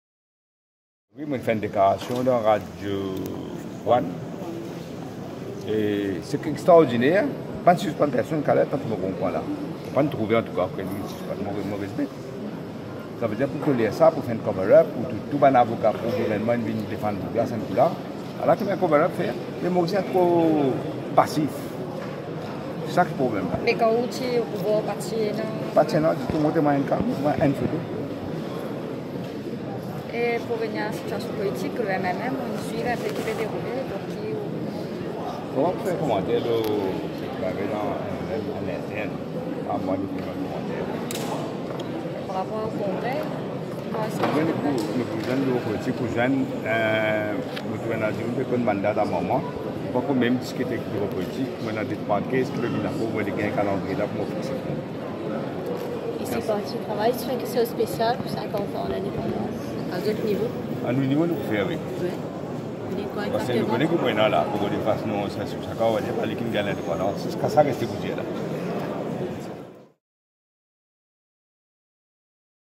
Navin Ramgoolam sur l'affaire David Gaiqui.
Lors du dêpot de gerbes à Pointe Canon, Navin Ramgoolam a commenté l'affaire Davin Gaiqui.